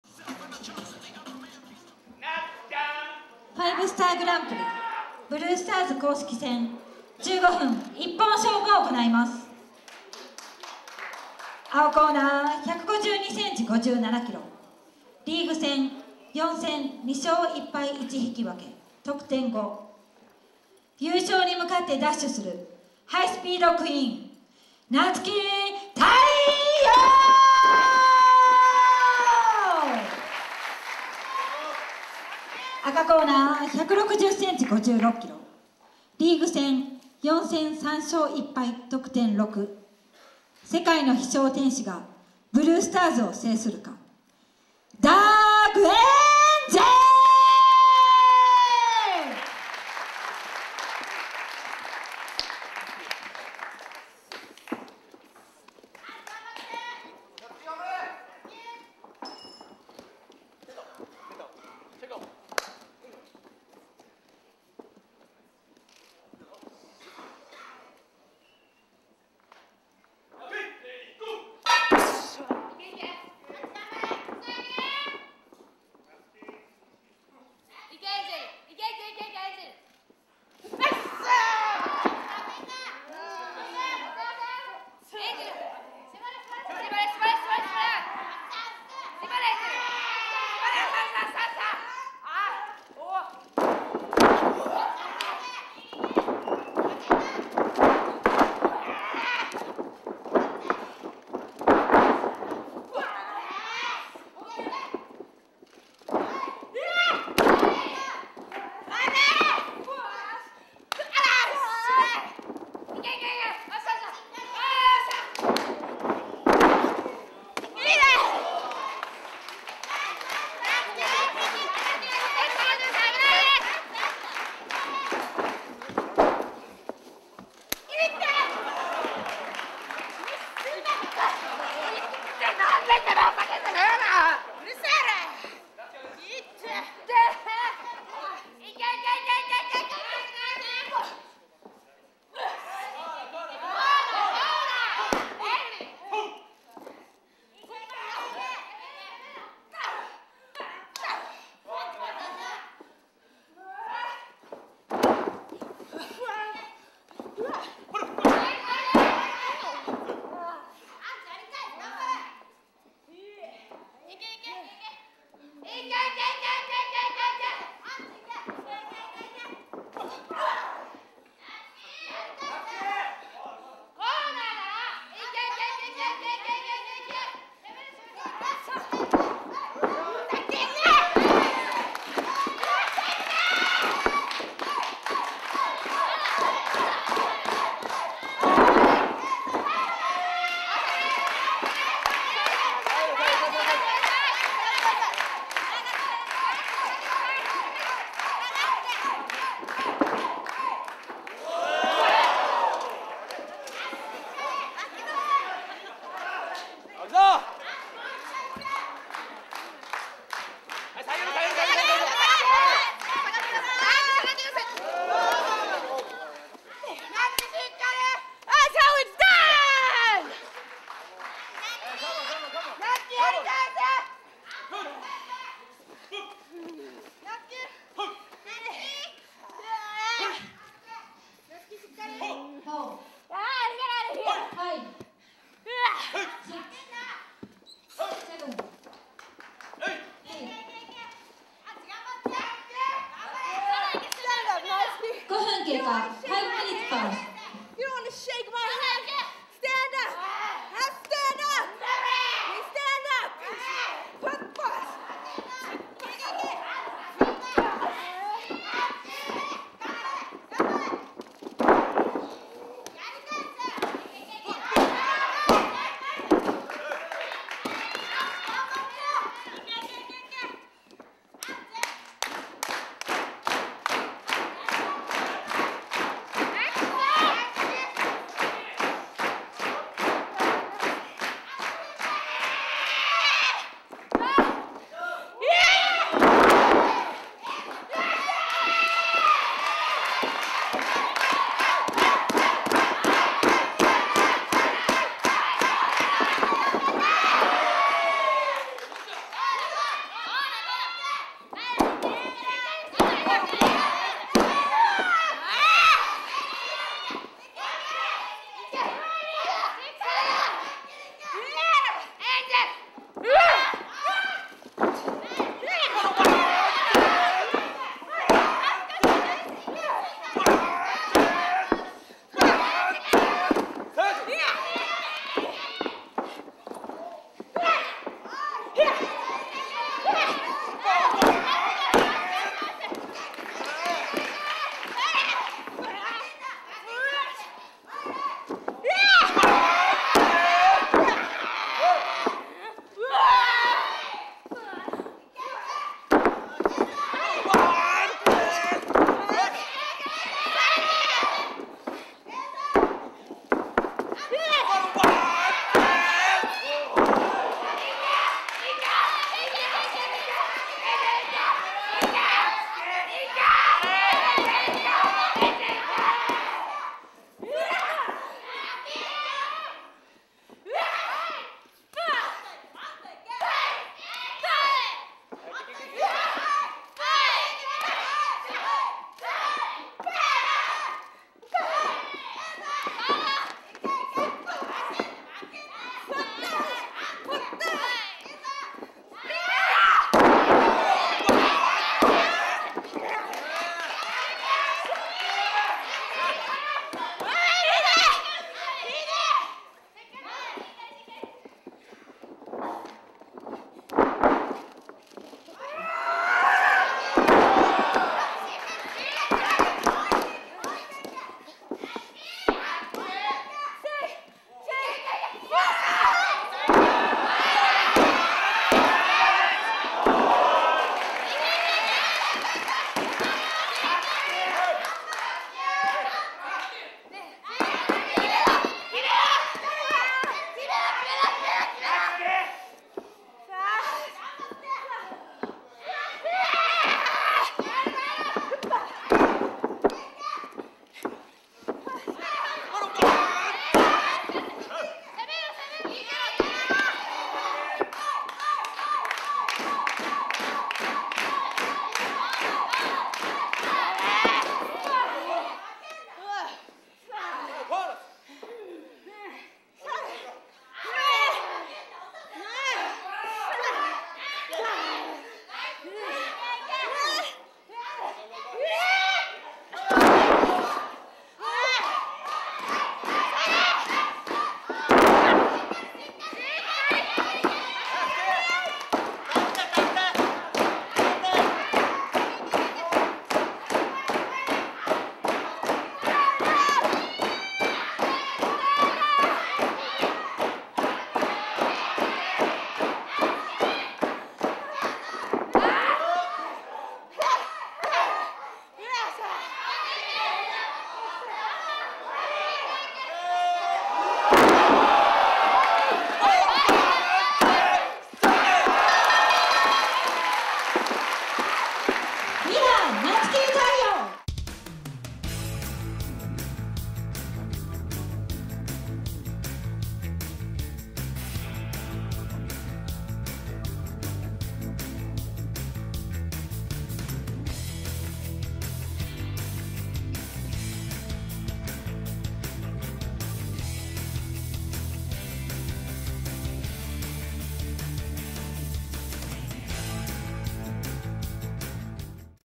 Dark Angel (Sarah Stock) vs. Natsuki*Taiyo
Dark Angel (Sarah Stock) vs. Natsuki*Taiyo from the Stardom 5* Stars GP tournament. Match was held Sept. 30th, 2012